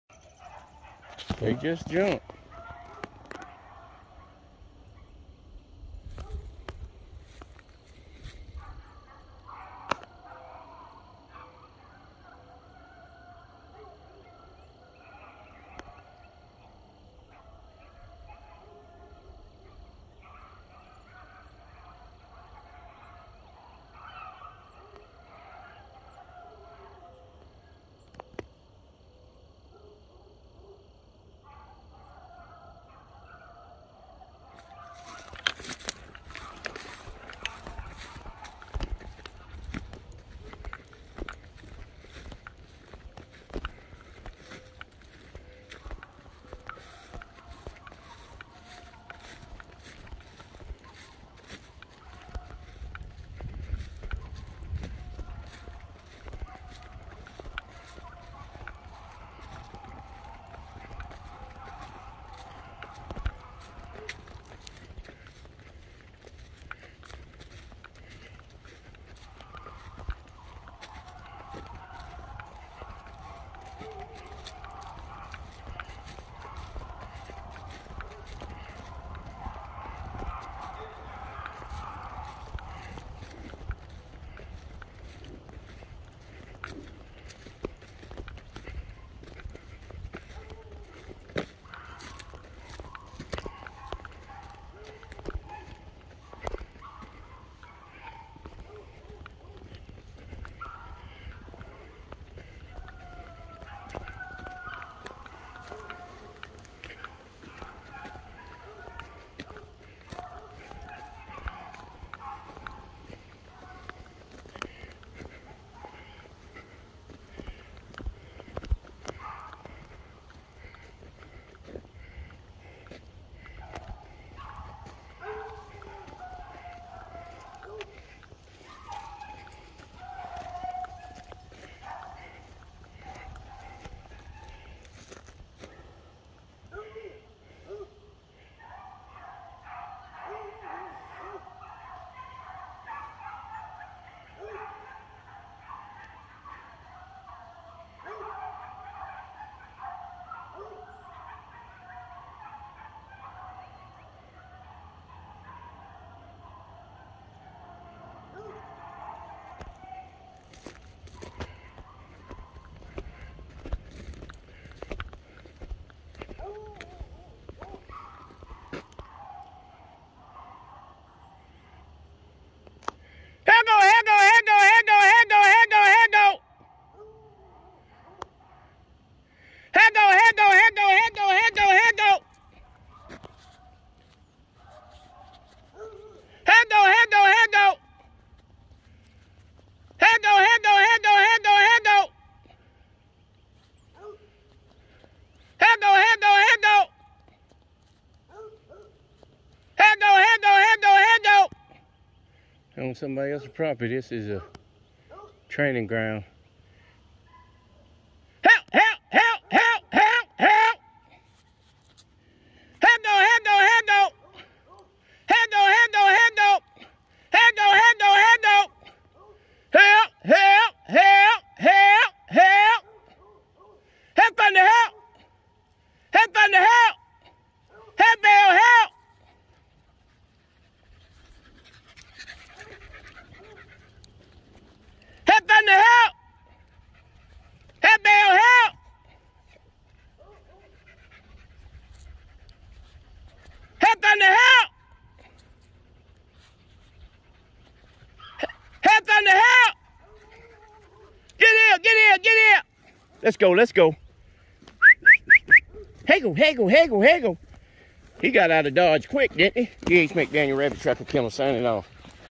Homeboyz Hunting Club with DHMCDANIEL Rabbit Tracker Kennel
Spring training 3 dogs 3 pups